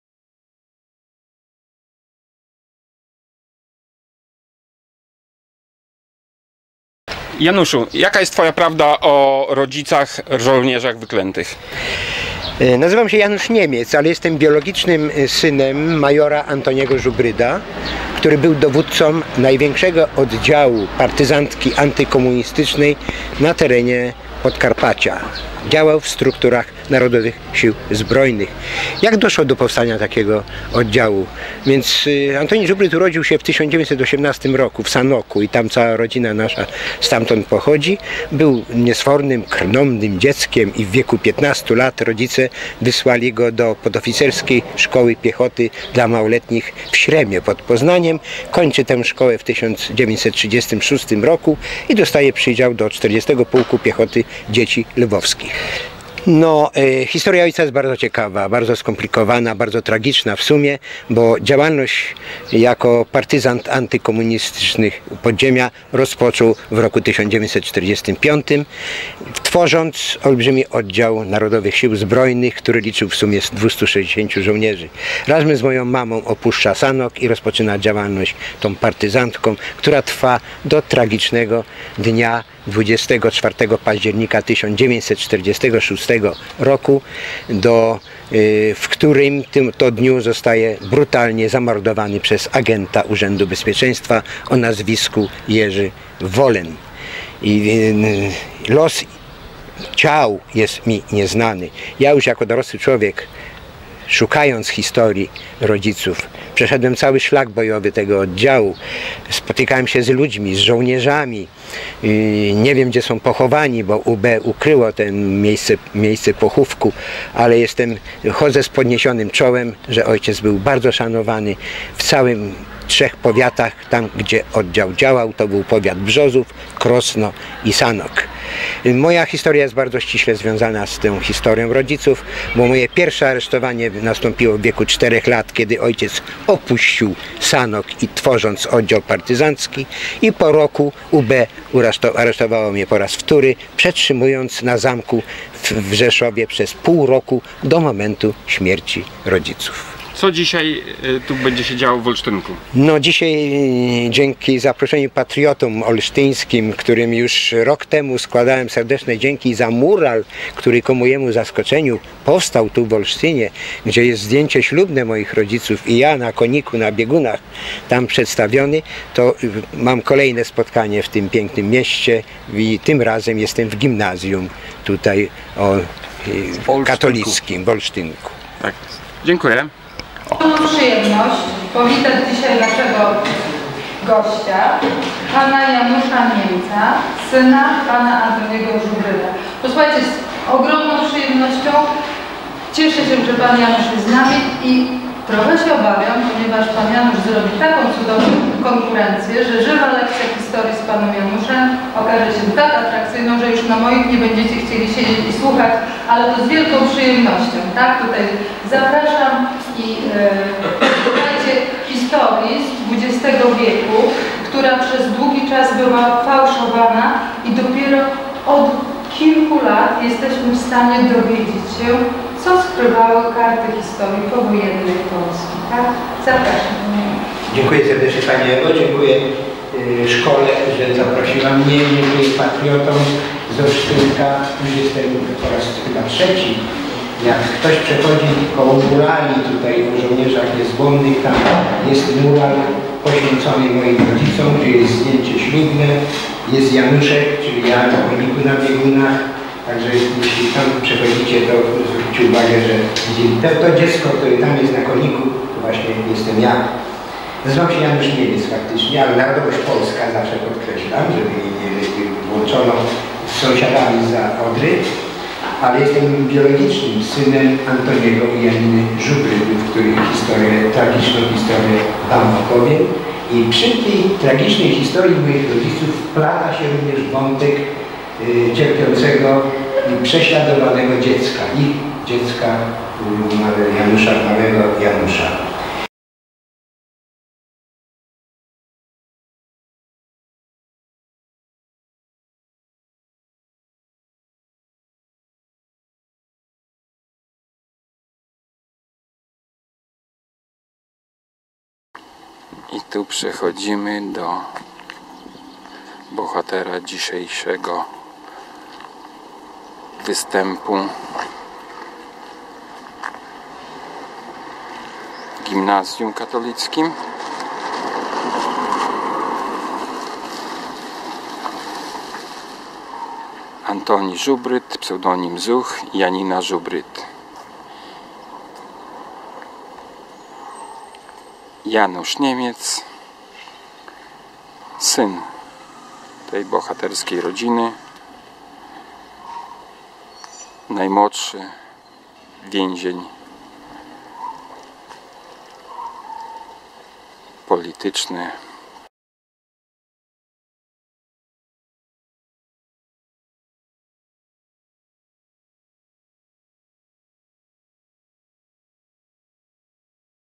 Major Antoni Żubryd był krnąbrnym i niesfornym dzieckiem! Janusz Niemiec - Olsztynek - 29.05.2017
Proszę o wsparcie niezależnego kanału i wolnego słowa KuPrawdzie​​​​​​​​​​​​​​​​​​​ na konto: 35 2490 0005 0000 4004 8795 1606. Wspomagających honoruję na